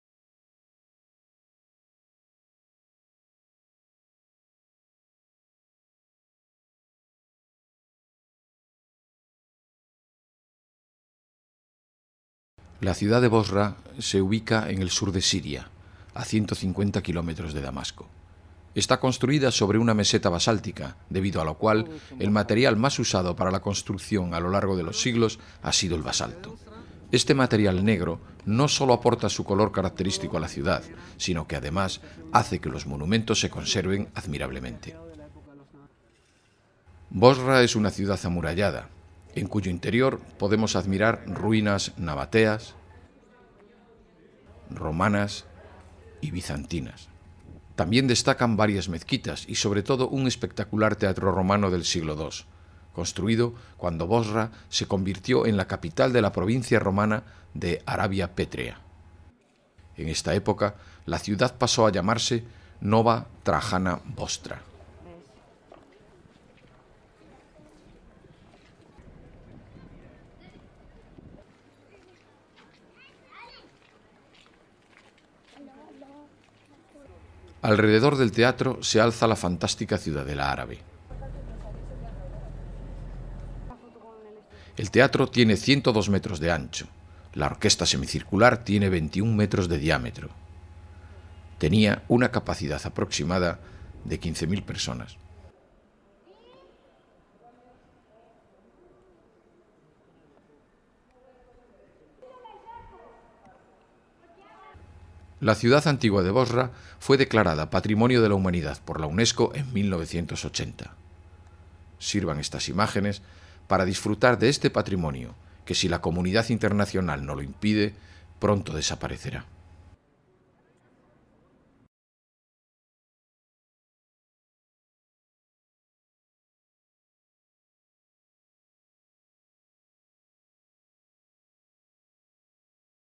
Siria. Bosra
La ciudad Siria de Bosra, declarada patrimonio de la Humanidad por la Unesco podría llegar a desaparecer por la guerra civil que en estos momento asola Siria.The Syrian city of Bosra, declared World Heritage Site by Unesco could eventually disappear by the civil war raging right now Syria.La ville syrienne de Bosra, déclarée patrimoine mondial par l'Unesco pourrait finir par disparaître par la guerre civile qui fait rage en ce moment en Syrie.